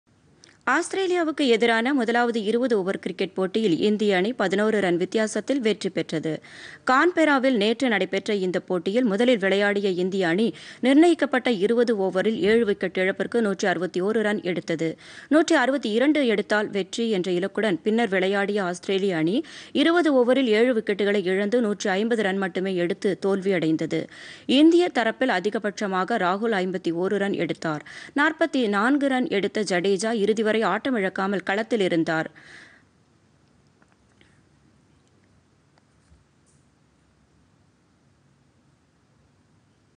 முதல் டி-20 கிரிக்கெட்: இந்தியா வெற்றி
ஆஸ்திரேலியாவுக்கு எதிரான முதலாவது 20 ஓவர் கிரிக்கெட் போட்டி - 11 ரன் வித்தியாசத்தில் இந்தியா வெற்றி